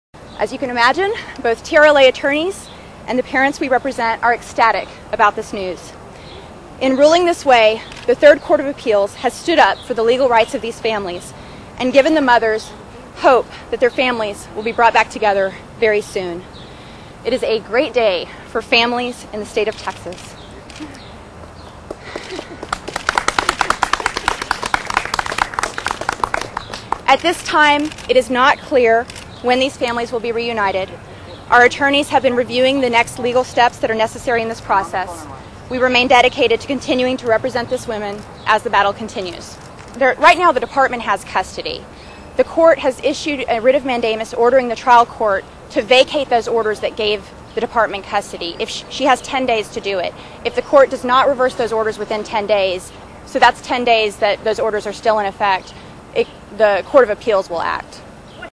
Lawyer: A Great Day for Texas Families
PlusLawyer: A Great Day for Texas FamiliesLawyer: A Great Day for Texas FamiliesThe Associated PressA lawyer representing dozens of women whose children were taken from them is applauding a Texas appellate ruling, that states child welfare officials had no right to seize more than 400 children living at a polygamist group's ranch. (May 22)This video contains ONLY natural sound. No script is available.